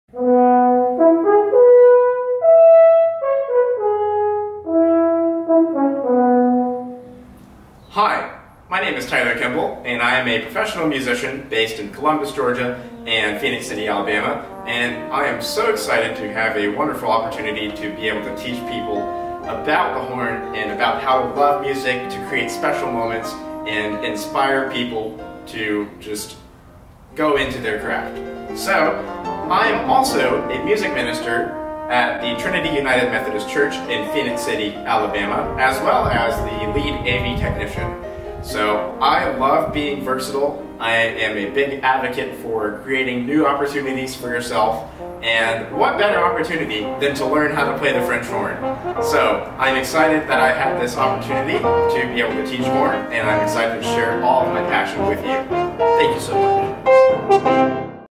About Tyler Kimball
I’m a versatile professional French Horn player based in Columbus, Georgia. I play 2nd Horn in the Albany Symphony Orchestra based in Albany, Georgia and I’ve performed with the Columbus Ballet. My primary teachers include Achim Reus from Athens Georgia, and Dr. Natalie Brooke Higgins, and Dr. Anne-Marie Cherry at the Schwob School of Music at Columbus State University in Columbus, Georgia. I’m a general freelancer and travel all around Georgia and Alabama. I’m a Music Minister and A/V director at Trinity United Methodist Church in Phenix City, Alabama. I am an amateur photographer/ videographer.